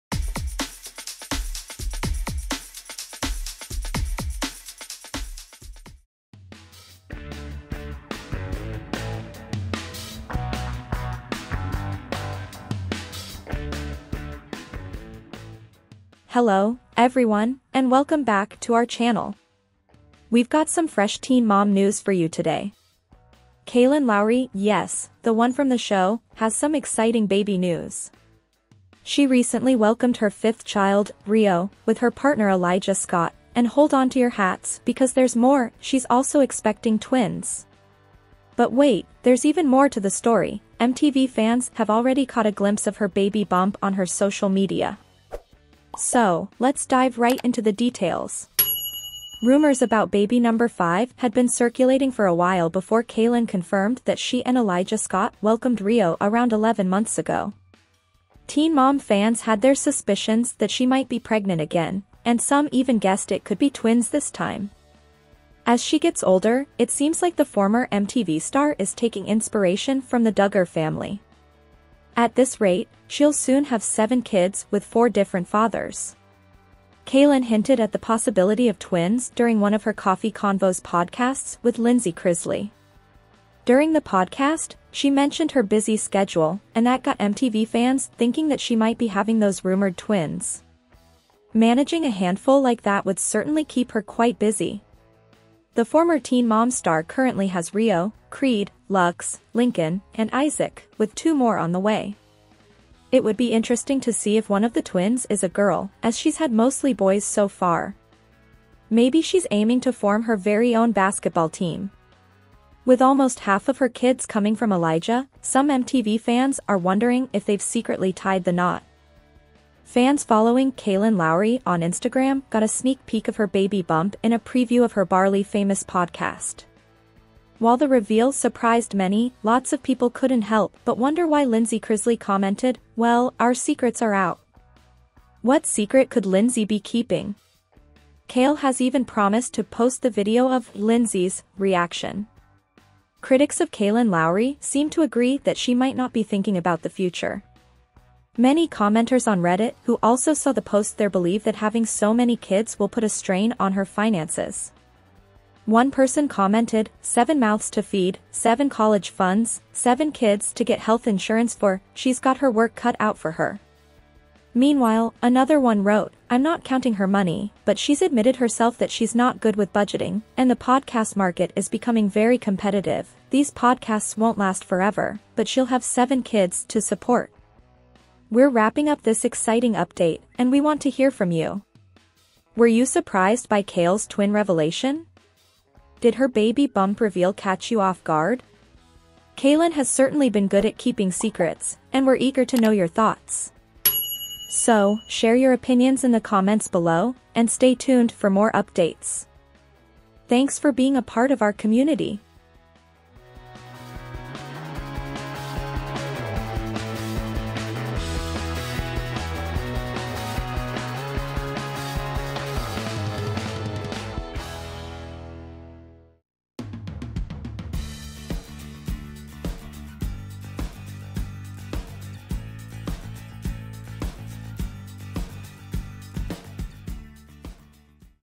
EXCLUSIVE!!! 'Teen Mom' Kailyn Lowry Flaunts Her Twins’ Baby Bump!
We've got some fresh Teen Mom news for you today. Kailyn Lowry, yes, the one from the show, has some exciting baby news. She recently welcomed her fifth child, Rio, with her partner Elijah Scott, and hold on to your hats because there's more – she's also expecting twins! But wait, there's even more to the story; MTV fans have already caught a glimpse of her baby bump on her social media. So, let's dive right into the details!